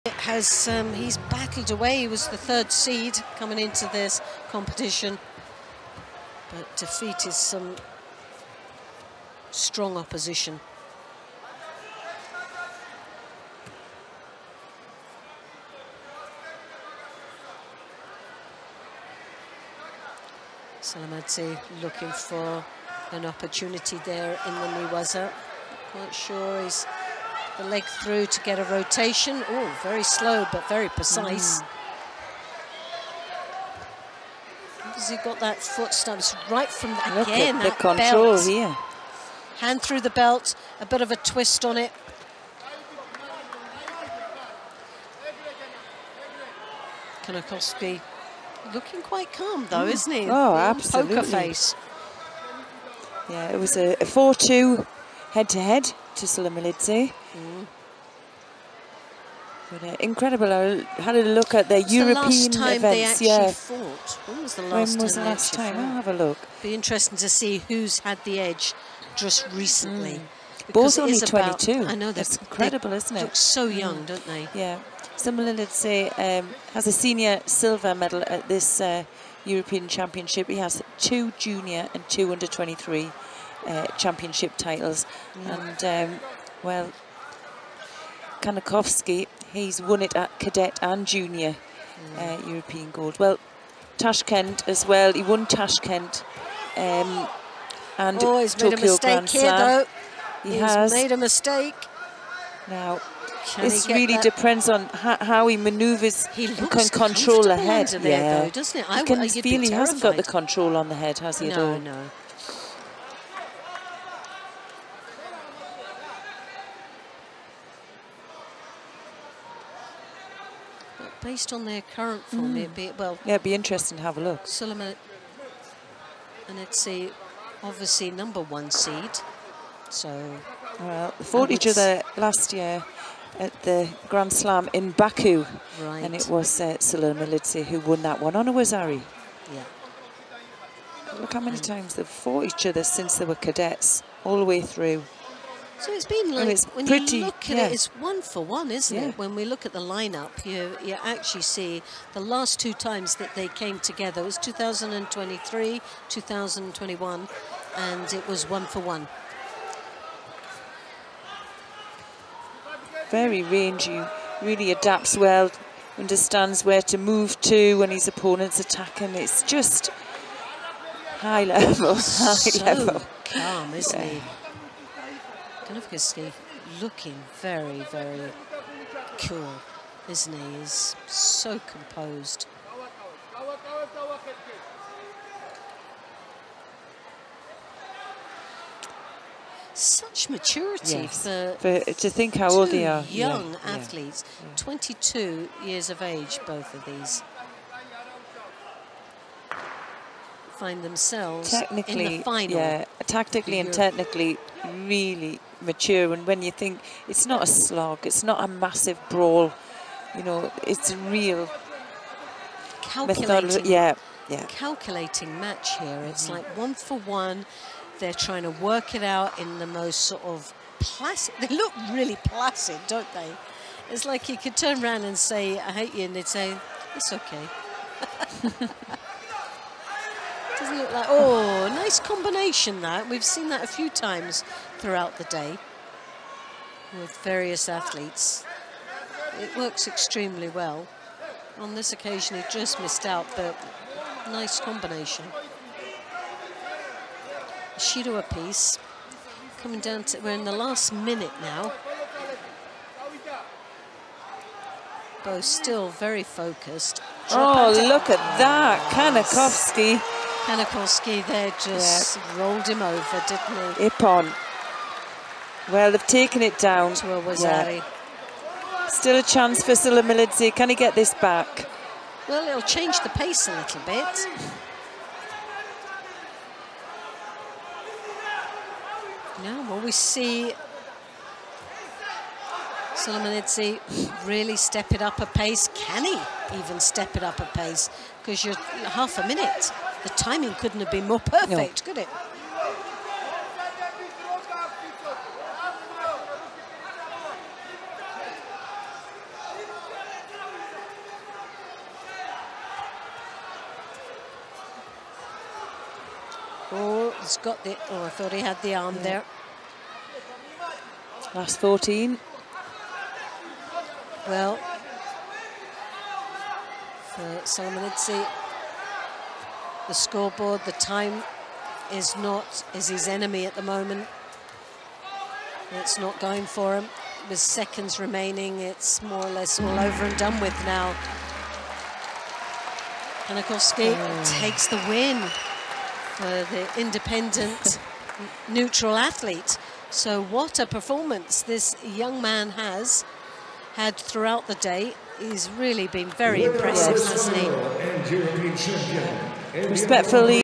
Ilia Sulamanidze V Matvey Kanikovskiy | European Championships 2024
2024 European Judo Championships Seniors Individuals, 100 Final - Ilia Sulamanidze (GEO) V Matvey Kanikovskiy (AIN)

🎥 | Credits: EJU